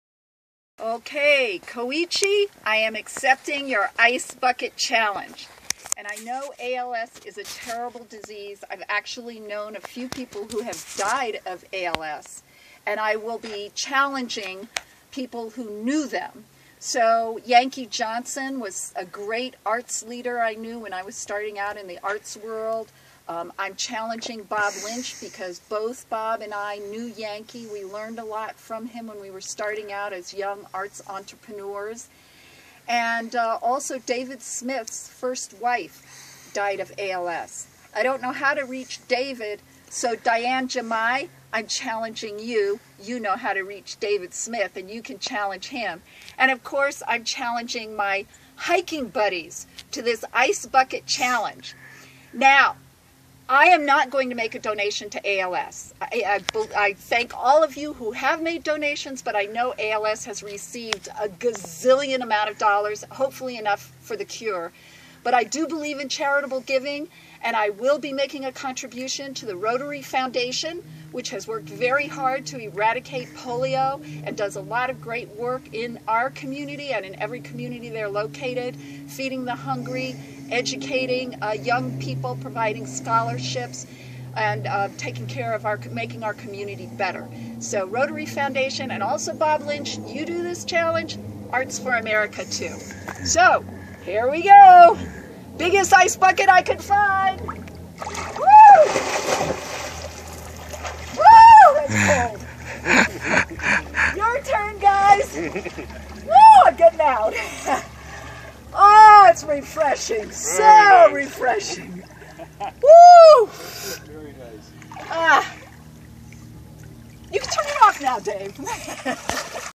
Ice bucket challenge
Wendy Ceccherelli at Rachel Lake, Cascade Mountains, WA